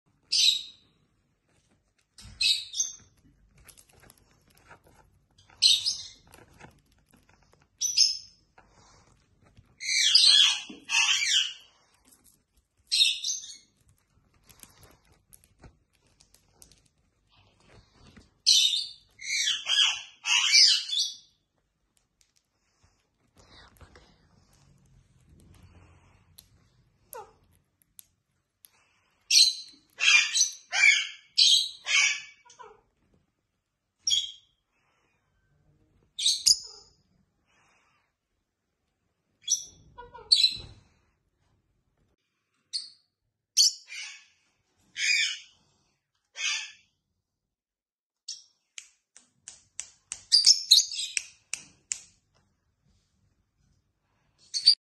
Lovebird talking 😍 | tame lovebird |Lovebirds talking and singing
Lovebirds is one of the most beautiful lovebirds that Friendly  with human.In this video,you will see them are feeding and playing with toy.Feeding send seeing then eating the food is one of the relaxing bird watching at home.